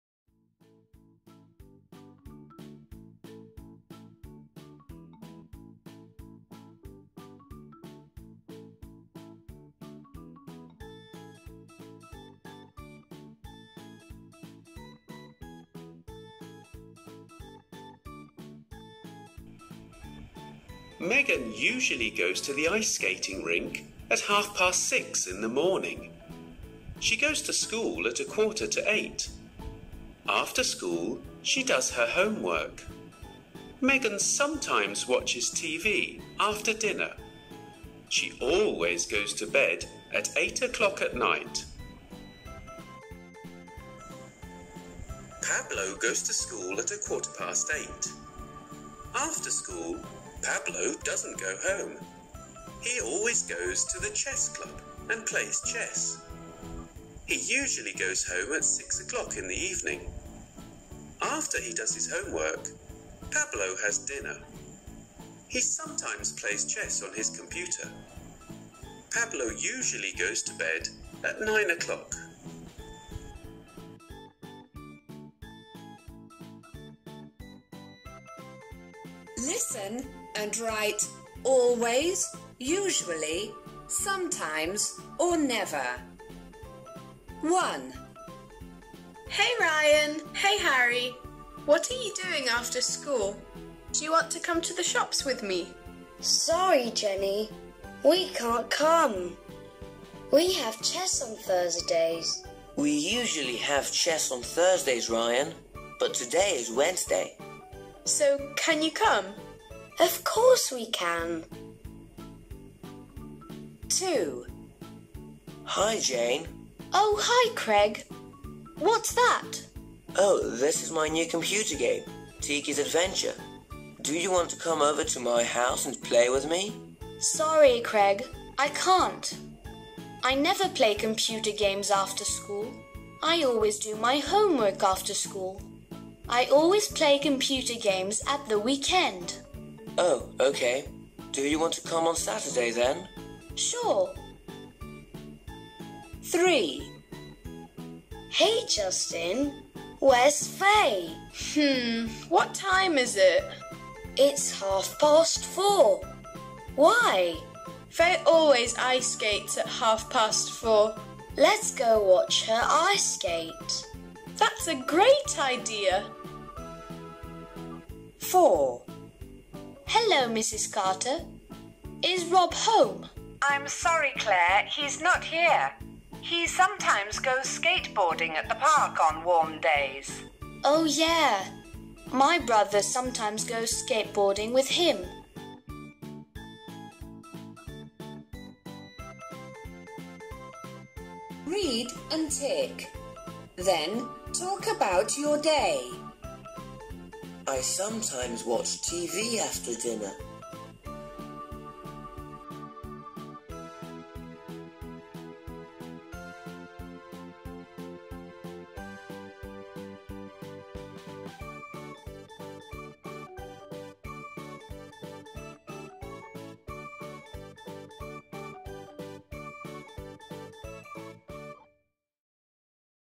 Get Smart Plus 4/ 1 OUR WORLD
This video covers activity 3 and 4 by listening to the previous lesson.